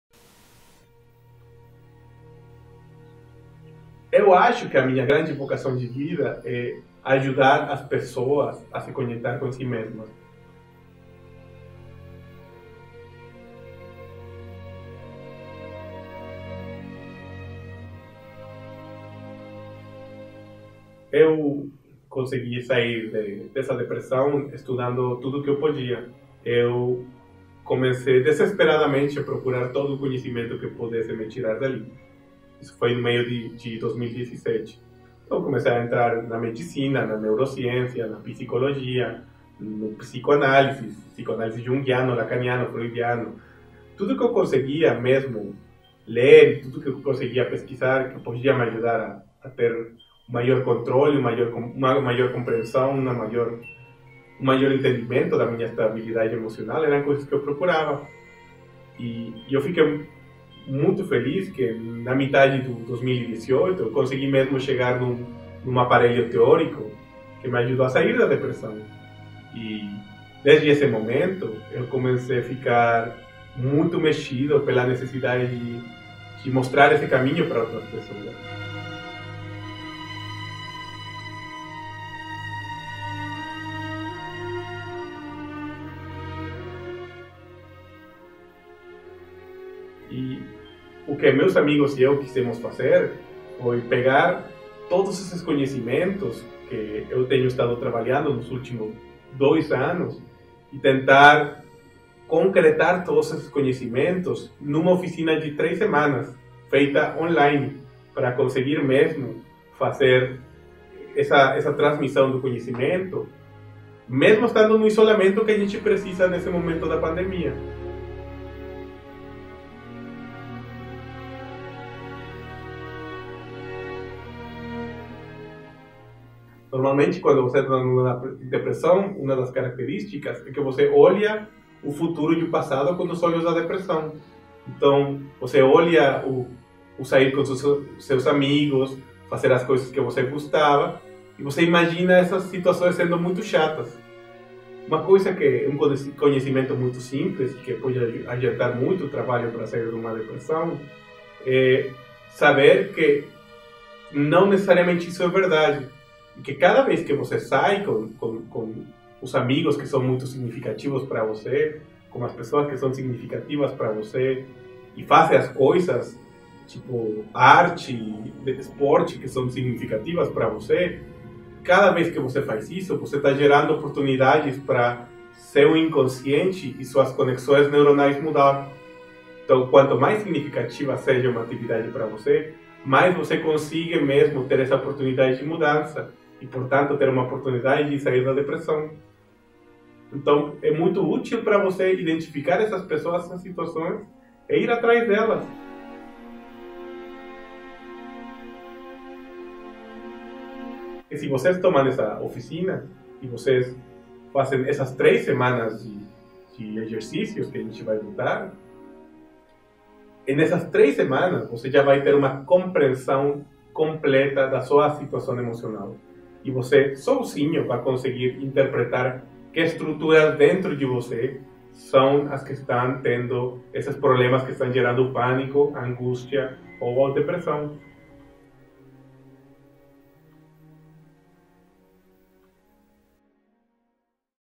Inscrições abertas pro Workshop Autopsicanálise para sair da Depressão
“Workshop intensivo online de Autopsicoanálise para sair da Depressão: controle do pânico, angústia, medo e ansiedade”.

Produzido por "Proyecto Humano": Julián Bastidas Treviño (criação e direção da teoria) e Diego Meillón (criação e edição audiovisual); com o apoio inicial de Joe Rodríguez.

Facebook: Julián Bastidas Treviño ou Diego Meillón.

Datas: de segunda-feira 10 de agosto à segunda-feira 31 de agosto de 2020.
3 semanas ou 22 dias, trabalho diário.

Custo: 400 reais pelo Workshop completo.
Bolsas de 25%, 50% e 90%*.

*Para a bolsa é preciso escrever uma carta ao e-mail de contato especificando a razão e qual a bolsa solicitada

Dados para depósito:

Ag: 2801-0
C. poupança: 88.143-0
Variação: 51
Aline Binns de souza e Oliveira

Para confirmar sua inscrição envie mensagem, com o comprovante do pagamento, aos organizadores.

Sobre o Workshop:

Quando a tristeza nos aprisiona o constante vazio que sentimos as vezes se faz tão intenso que acreditamos que não há nenhuma saída. Não é assim. Em nenhum caso, nunca tem que ser forçosamente assim: isto não é nunca definitivo.
Haveremos de recordar sempre que em nosso passado existe ao menos uma história, de algum tempo na vida em que fomos plenamente felizes. Inclusive se isso aconteceu somente por certos períodos na infância: aconteceu, sim aconteceu. Esta é a esperança, é saber que se pode estar de outra maneira, que existe outro modo.

Este workshop está alimentado por vivências pessoais , bem como por uma síntese de distintos conhecimentos provenientes da psicología, da psicoanálise, das neurociências, da biología, da medicina e da filosofia. Ambos, tanto Diego, que se encarrega do audiovisual, como eu, Julián, que estou dedicado a configuração teórica e prática do conteúdo, já temos vivido crises emocionais profundas. Elas se convertem hoje em um motor que nos é essencial para fazer construir a totalidade do que cimenta este Workshop e também o Proyecto Humano (dedicado a construir materiais dessa natureza).

Eu, por exemplo, vivi uma depressão horrível de 2 anos que quase me tirou a vida em um de seus pontos. Estas duras vivências me levaram, sendo artista de profissão e maestro em Filosofia e Ciências Sociais, a desenvolver métodos capazes de sintetizar as ferramentas acessíveis tanto de psicanálise, como da psicologia, medicina, neurociências e biología para que eu saísse da depressão. 2 anos depois dessa experiência se fez parte muito primordial de minha vida  dedicar-me a compartilhar esses resultados com outras pessoas. Considero que minha vocação principal na vida é ajudar pessoas a se conectarem com quem são à partir de si mesmas e a empoderar-se desde dentro.

Neste workshop o que faremos acima de tudo é compartilhar um método de compreensão e reconstrução interna, que leva o nome de "Autopsicoanálisis": que significa um conhecimento capaz de dialogar com nosso interior de forma profunda e terapêutica. Por isto se pretende que quem assista estas três semanas seja capaz de fazê-lo sem a necessidade obrigatória de passar por um intermediário: psicólogo, psicoanalista, psiquiatra ou terapêuta. O que se pretende é dar esse empoderamento consciente a todas as pessoas que participem; no entanto, eu não poderia deixar de dizer que o conteúdo deste workshop não está em conflito com os processos que a pessoa esteja vivendo.

Forma de construção do conhecimento:

O workshop consiste de 3 semanas de exercícios para realizar e conteúdo audiovisual proporcionado diariamente por meio da plataforma do Google Classroom (será preciso dedicar em média 2 horas de trabalho diário durante o Workshop).
Ele se complementará com um diálogo constante com o assessor do grupo, Julián, que dirigirá e se responsabilizará por todo o processo.

Aqui estão alguns vídeos do primeiro jeito em que eu compartilhei esses conhecimentos (em espanhol):